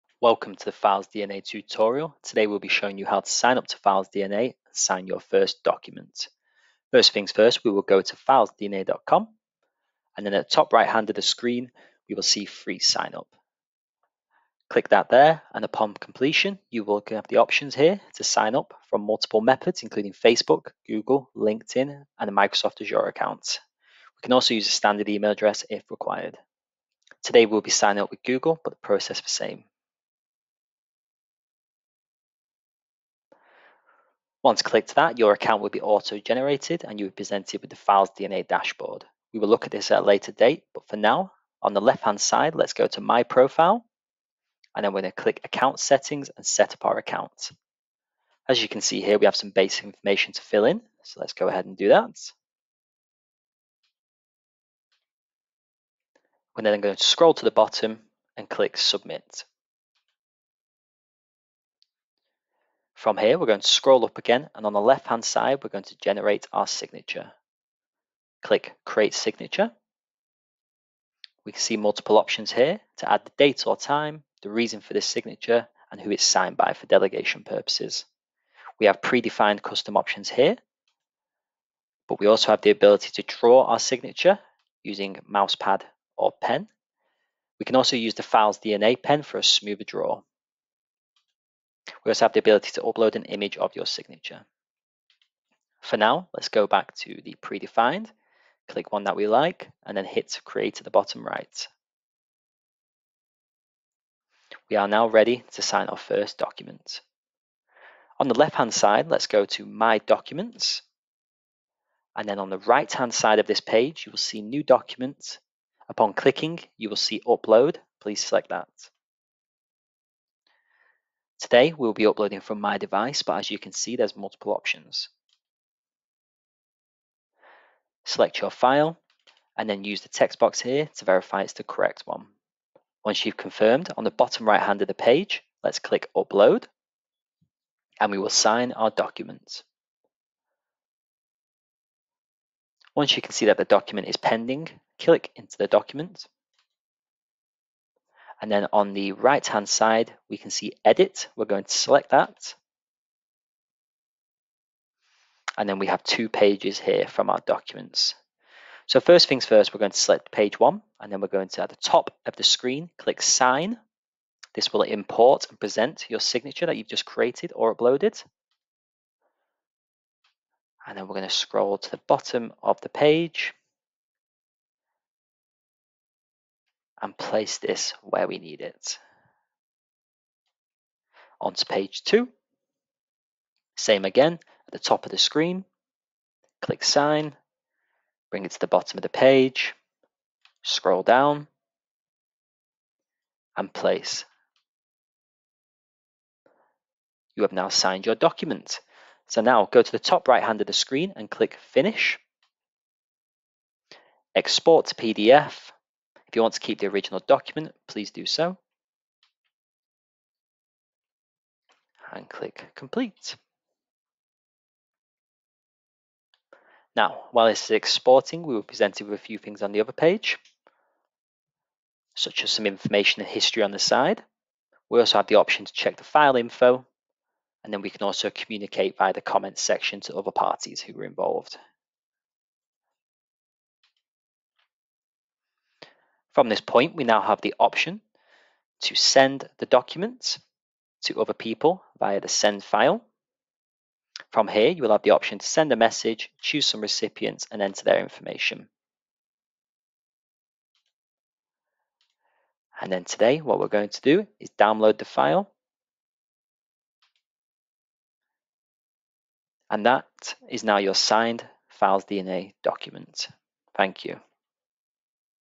How To Sign Document | Contract Paper | Sales Agreement Digitally With FilesDNA eSignature Software?
In this step-by-step tutorial, learn how to sign documents | contractpapers | salesagreements | PDF | wordfile digitally with FilesDNA esignature software on your PC & Laptops.

0:00 Welcome To The FilesDNA Tutorial
1:05 Create Signature
1:44 Ready To Sign The First Document

🎬Want to watch again or more videos on digital signature? Check out the other videos on our YouTube channel.